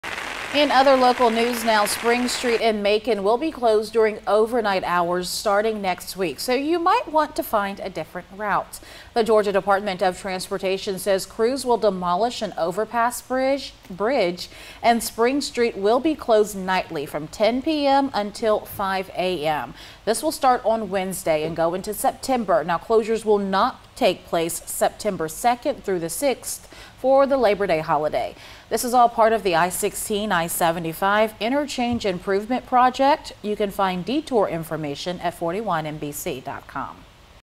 GDOT set to close Spring Street for overpass bridge demolition
MACON, Georgia (41NBC/WMGT) — Spring Street will be closed overnight starting next week as an overpass bridge is set to be demolished.